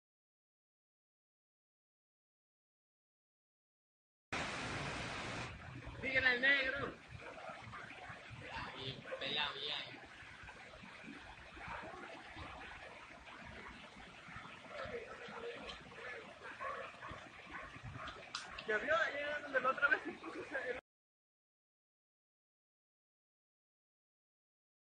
Ovni En Santiago Tuxtla Veracruz 3 De Diciembre 2020
miércoles 3 de diciembre 2020 se captó un ovni en Santiago Tuxtla Veracruz